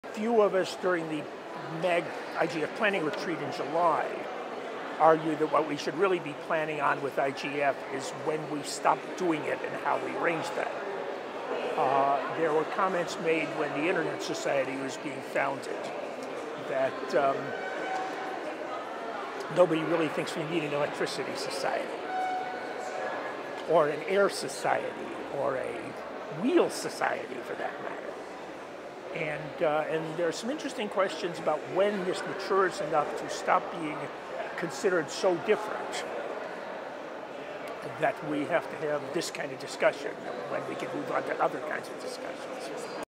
Global IGF 2016: John Klensin on Top Internet Issues
John Klensin, Principal, John C Klensin & Associates, shares his answer to the question, "What are the most difficult Internet challenges, fears or issues that must be addressed to assure the best future for all?" Recorded at the Global Internet Governance Forum 2016 in Guadalajara, Mexico, by the Imagining the Internet Center, an initiative of Elon University, North Carolina, USA.